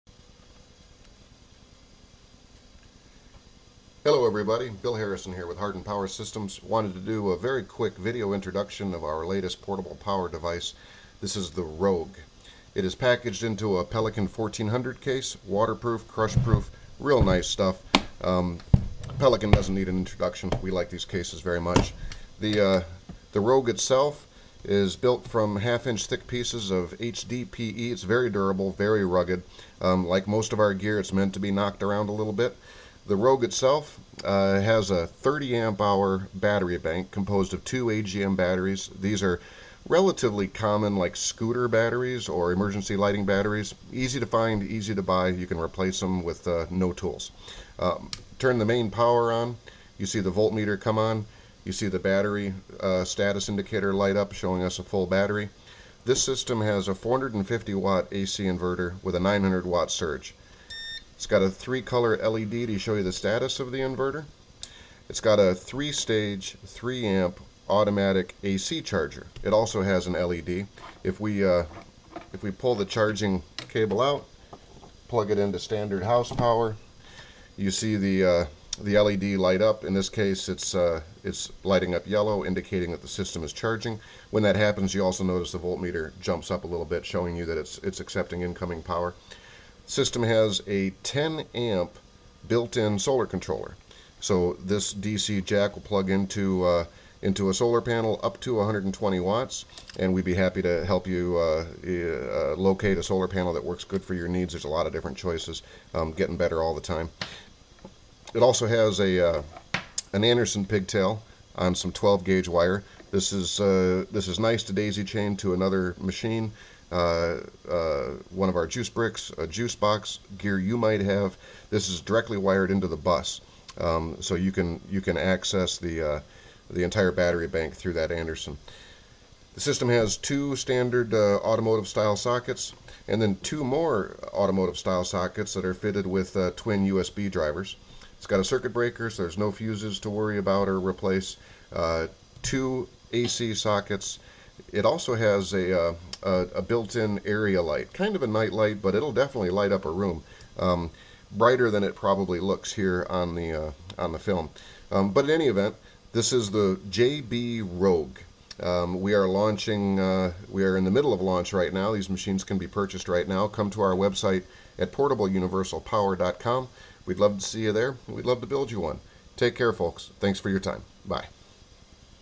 JB Rogue Flexible Power System
Very quick introduction of the new Rogue.  Built into a small Pelican case, the Rogue is a complete alt-energy system.  With a 450 watt inverter, automatic AC charger, 10 amp solar controller, and a beefy 30 AH battery bank, the Rogue is serious power to go.  Control panel has 4 USB ports, two cigarette sockets, Anderson Power Pole pigtail, and an LED area light.

and see this and other products.